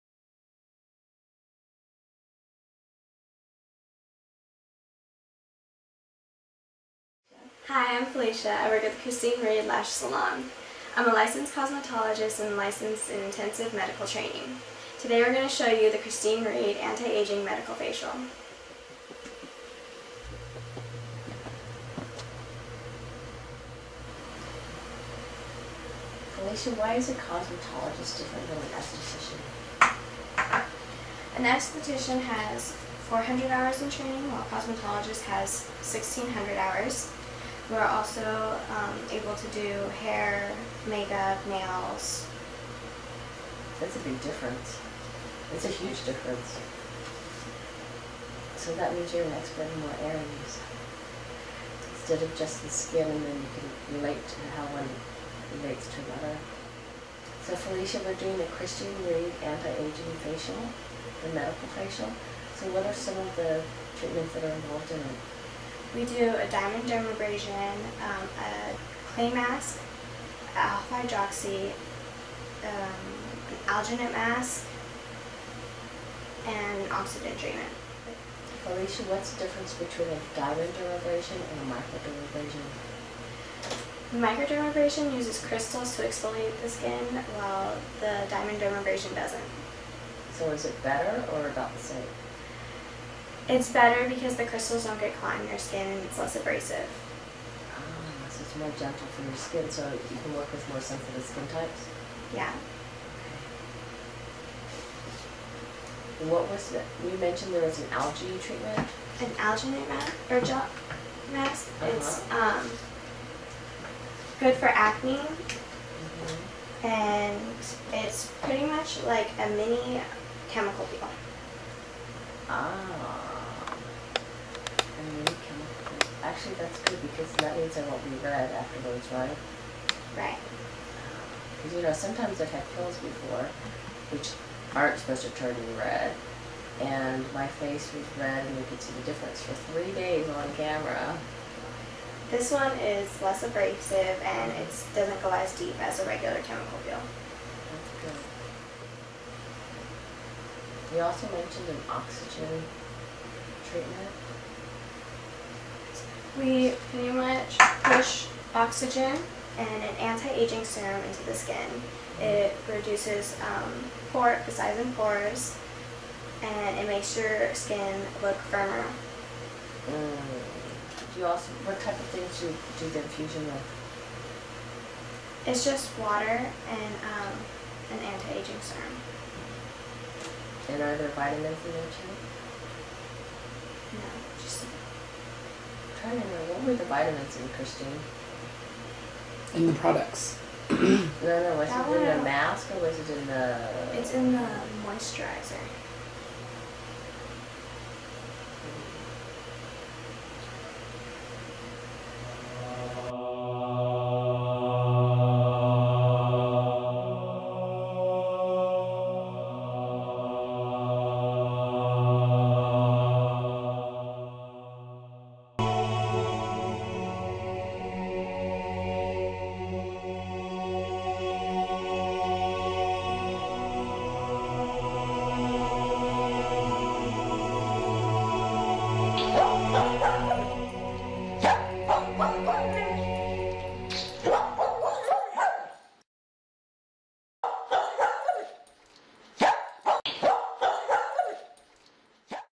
Anti-Aging Facial Questions and Demo
Cosmetologist vs., aesthetician, microdermabrasian vs. diamond dermabrasian and other questions answered while getting the ultimate of facials, the Christine Reed Anti-Aging Facial. Demo by Felecia Freeman, Christine Reed Lash Salon & Spa with Joyce Chow of MBN Newsvideoweb.